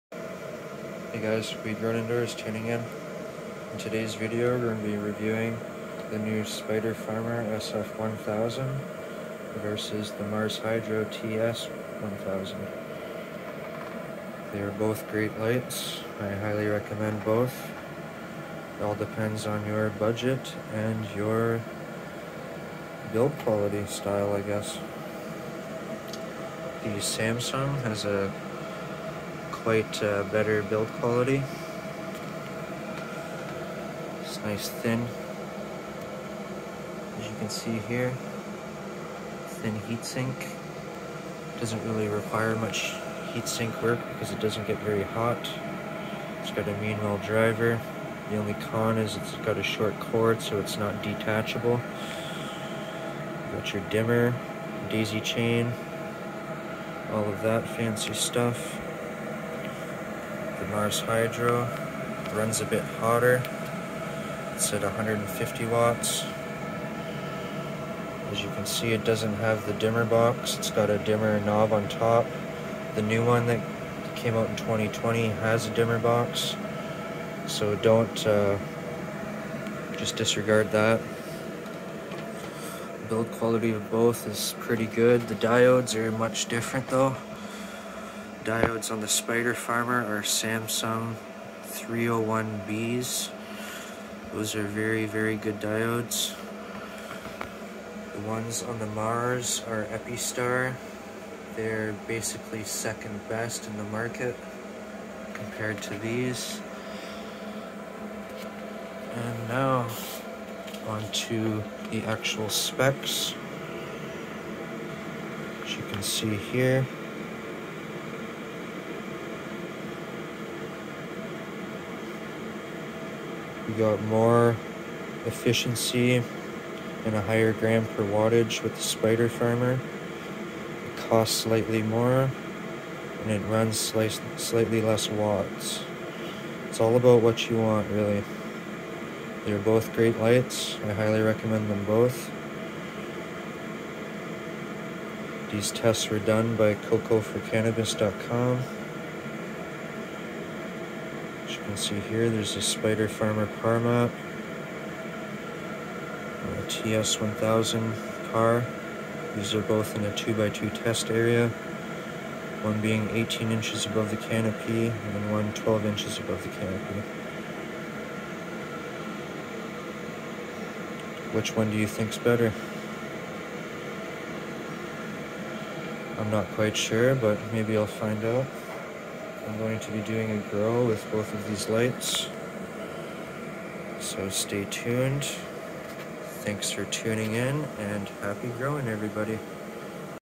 Spider Farmer SF1000 Vs Mars Hydro TS1000
In this video I compare the Spider Farmer SF1000 and the Mars Hydro TS1000 grow lights. I discuss the build quality and go over the specs and PAR map done by 'Coco for Cannabis'. Both of these lights are beasts with the full spectrum for plant growth and high efficiency. Which one is best?

HAPPY GROWING

This channel and its content are intended only for adults. I do not condone any illegal activities and this video was recorded for documentary purposes only.